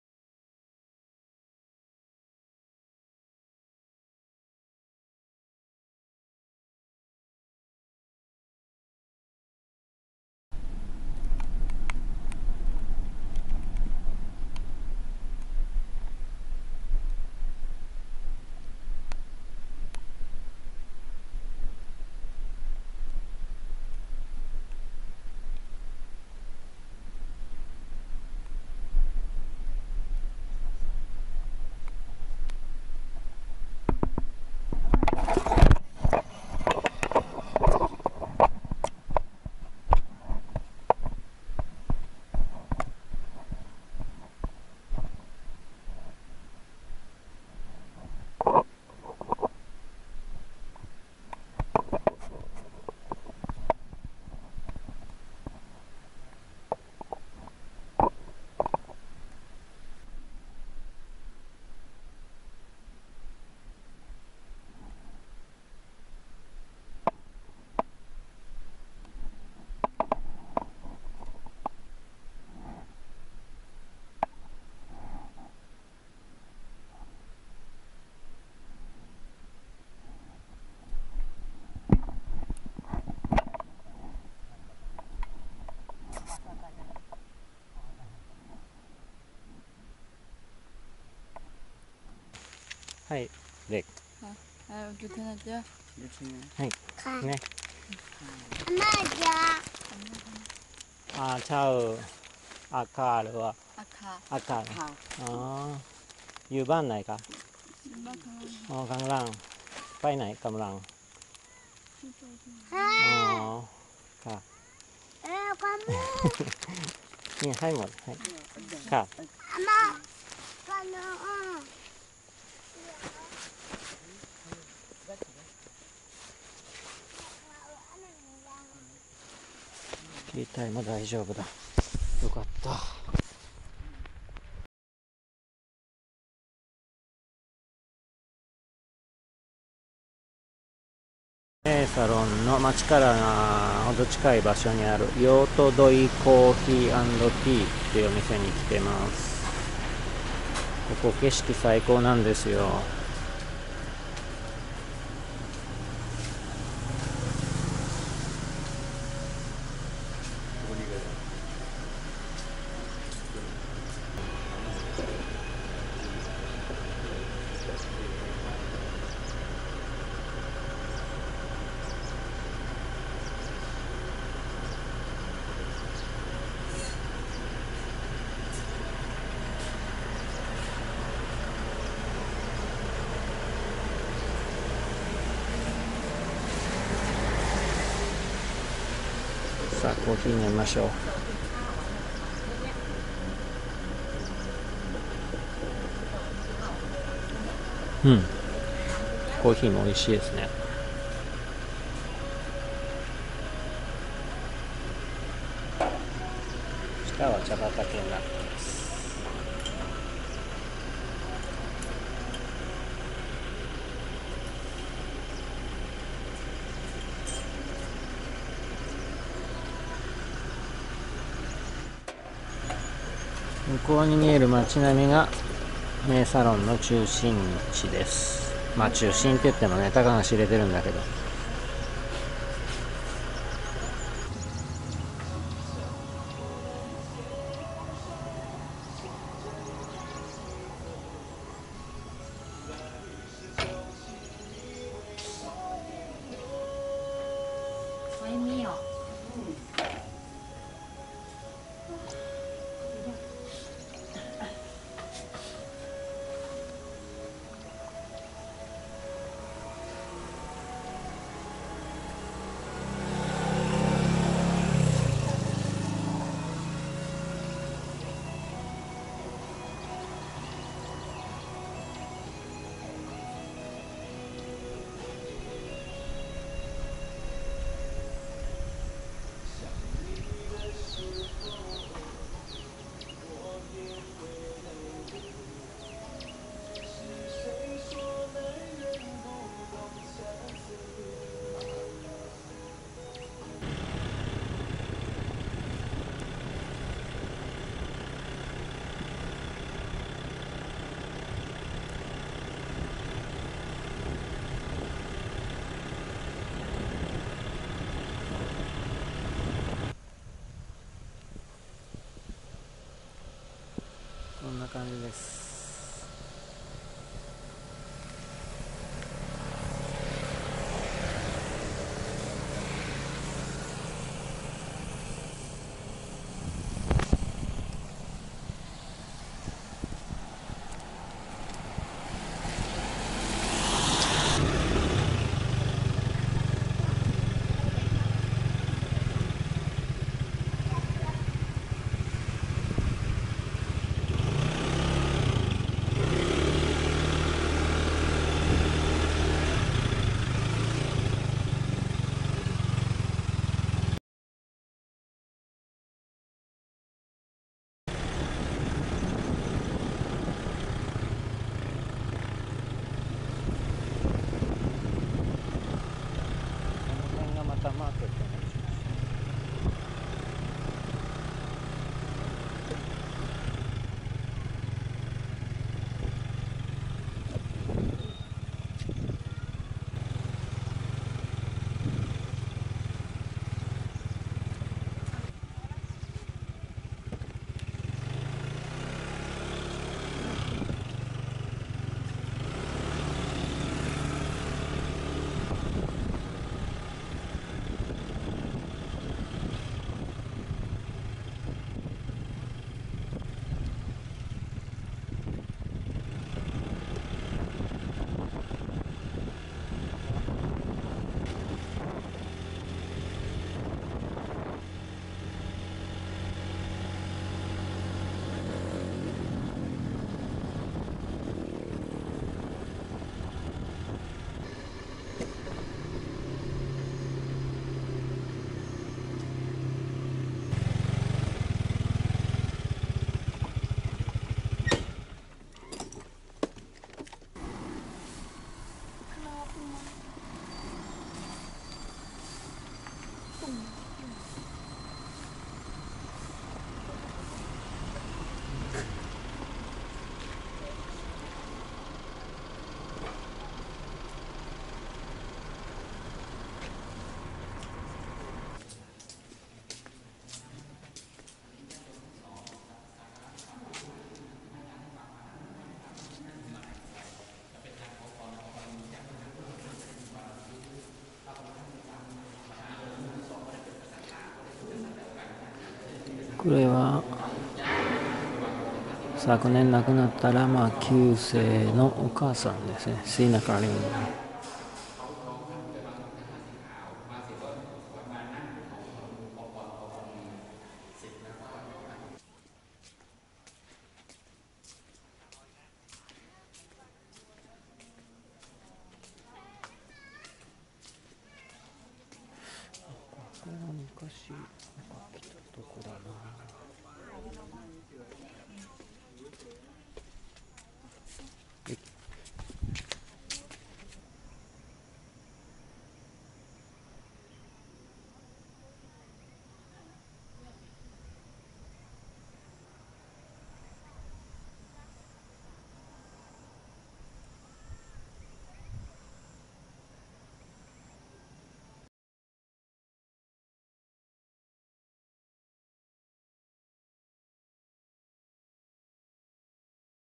バイクアクシデントと絶景カフェ | 北タイ一人旅2018EP15 | Bike accident and a cafe with a superb view in Mae Salong
旅の4日目。メーサロン滞在中。茶園を後にし、トゥアッタイという集落に向かう途中でアクシデント発生。すっかり自信を無くし、メーサロンに戻ることに。帰路、偶然見つけた景色の良いカフェでのんびりします。